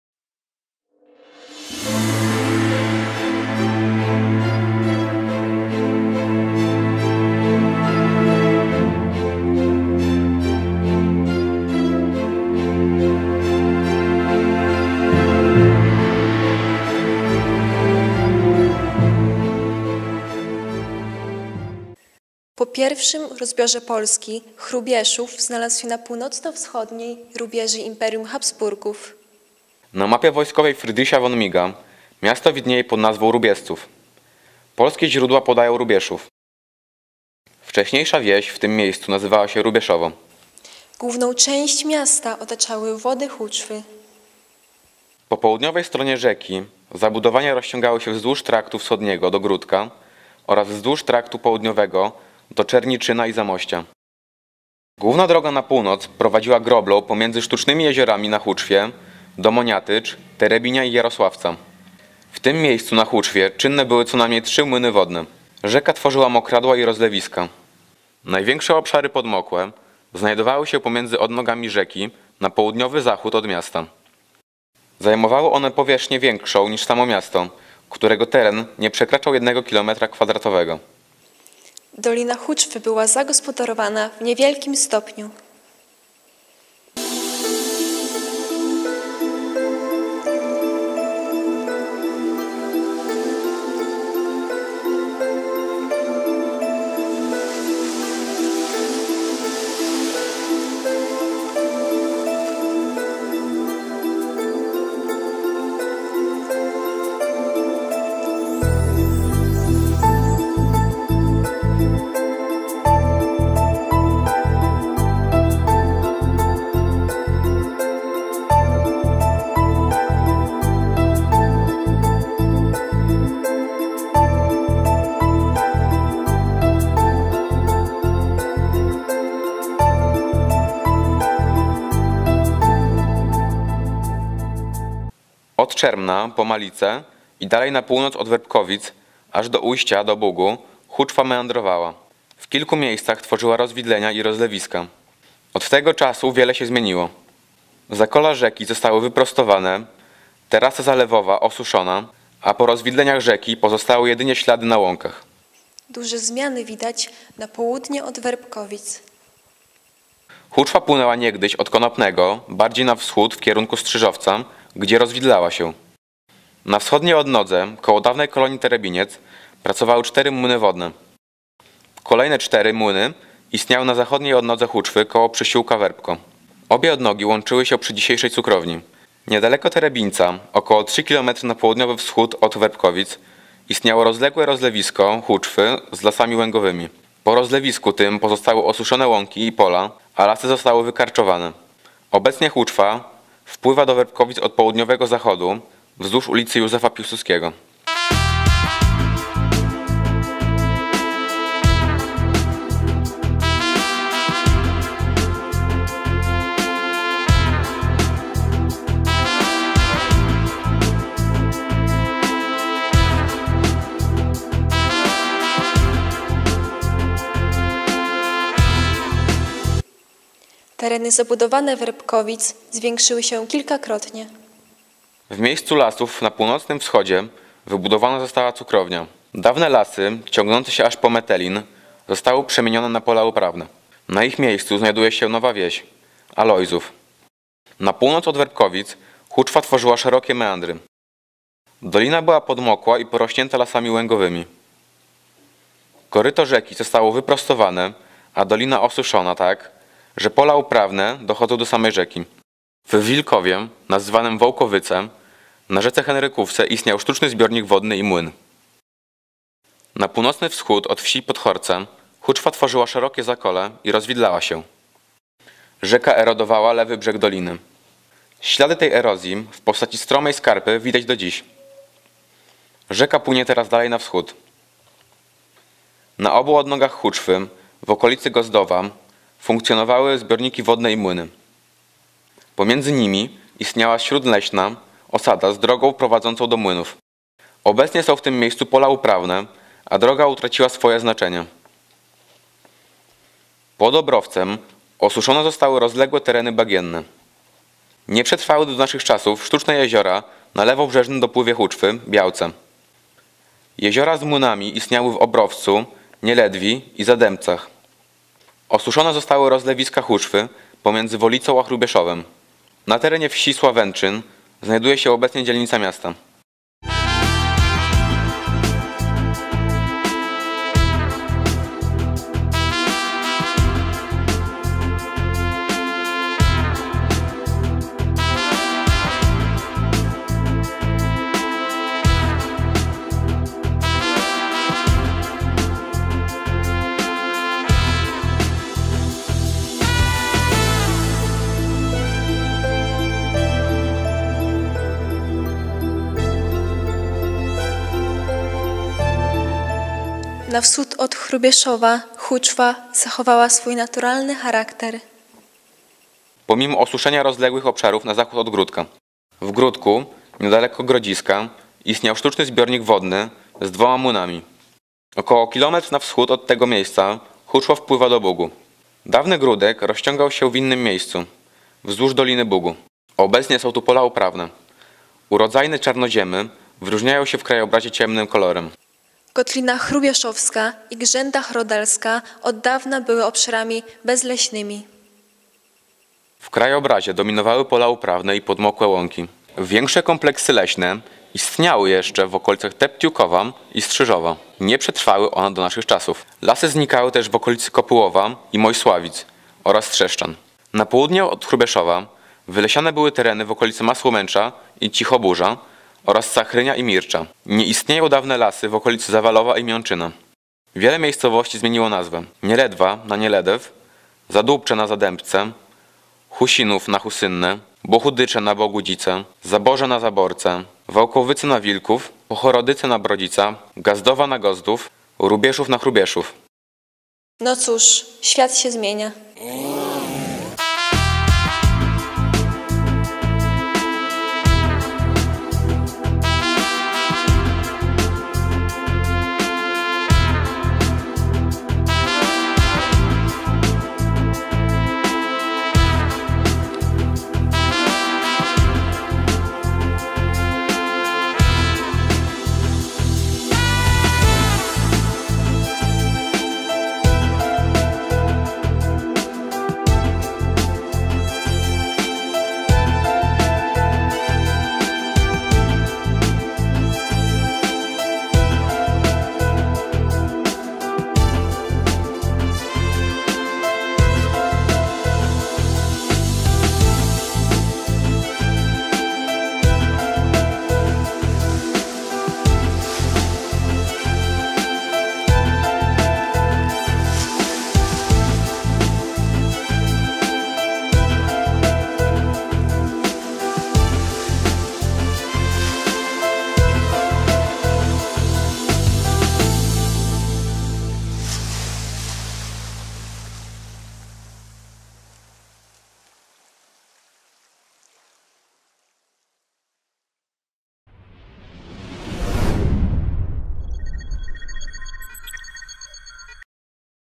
Świat sie zmienia (7) - Hrubieszów - Werbkowice
Film opowiada o tym jak wyglądały okolice Hrubieszowa ponad 200 lat temu. Co zmieniło się w dolinie Huczwy, w Werbkowicach oraz w Kotlinie Hrubieszowskiej w okolicach Gródka nad Bugiem